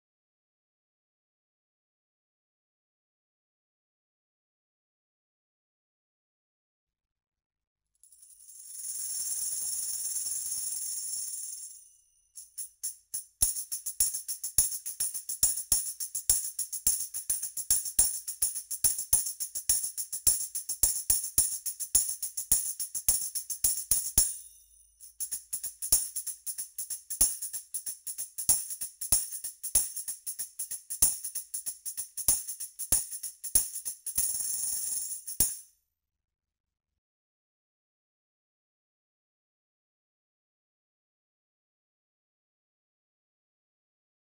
Meinl Tambourine CTA2M-BK | Sound Demonstration | How to play tambourine
Good sounds come in small packages! MEINL Percussion offers a complete line of Compact Tambourines. Light, fast, and full of sound, they are available with five different types of jingles.

The combination of steel and brass jingles allows today’s percussionist to add a special color that is both brilliant and warm to the music.

Meinl Tambourine CTA2M-BK features:

- Compact size
- Many jingle variations
- 2 rows of jingles
- Size: 8 " - 2 rows
- Material: Wooden frame, Steel, Solid brass